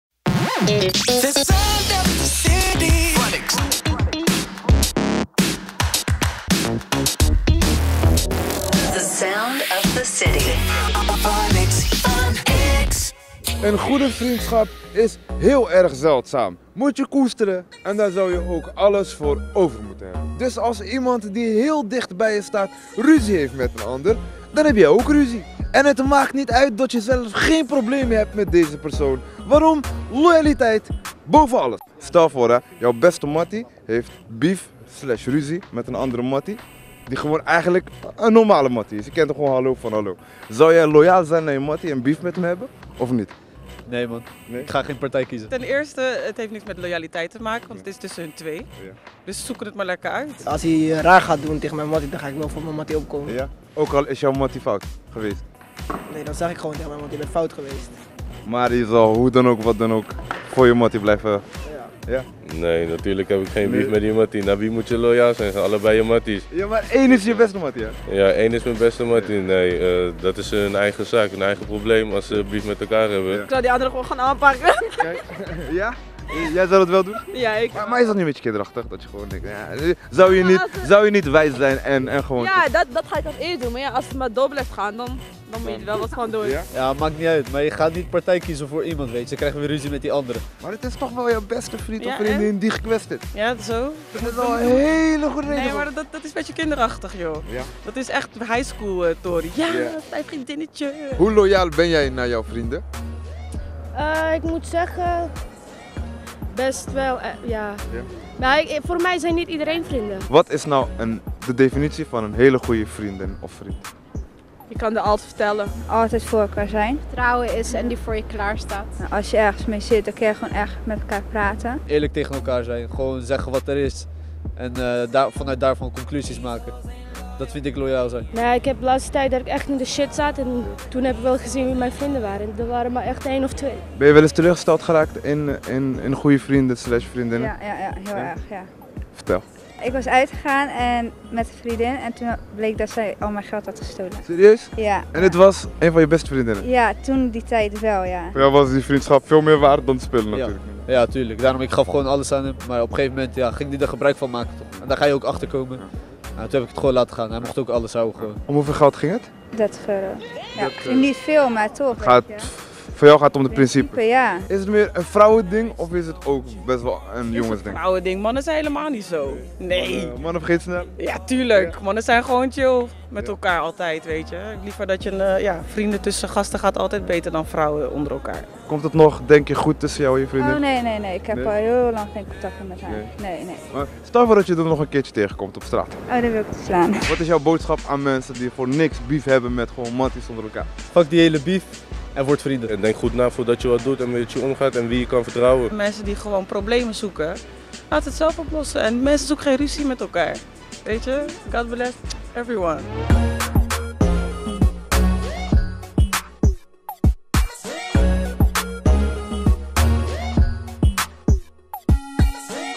Loyale vrienden
Een goede vriendschap is zeldzaam. Die moet je koesteren en daar zou je alles voor over moeten hebben. Dus als jouw beste vriend of vriendin ruzie heeft met een ander, dan heb jij ook ruzie met diegene. Loyaliteit voor alles! Toch?

FunX-reporter Yahia ging op onderzoek uit in Den Haag!